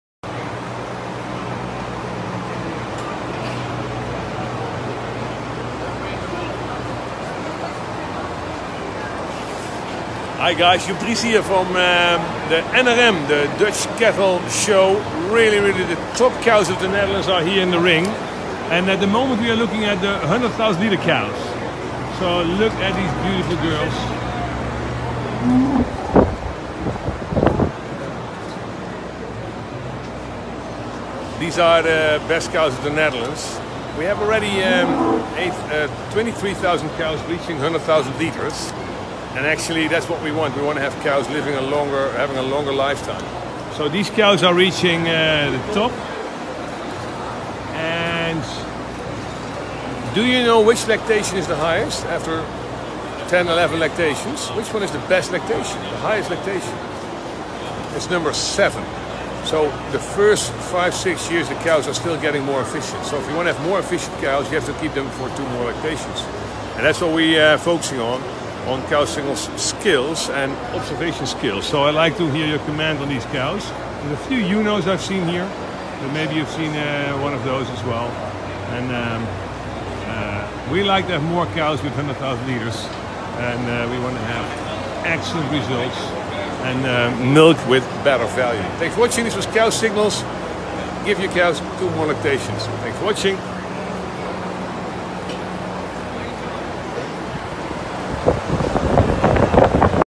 100,000 liter cows at NRM
These are the best cows in the Netherlands! This is what we want; top producing cows that live long and healthy productive lives.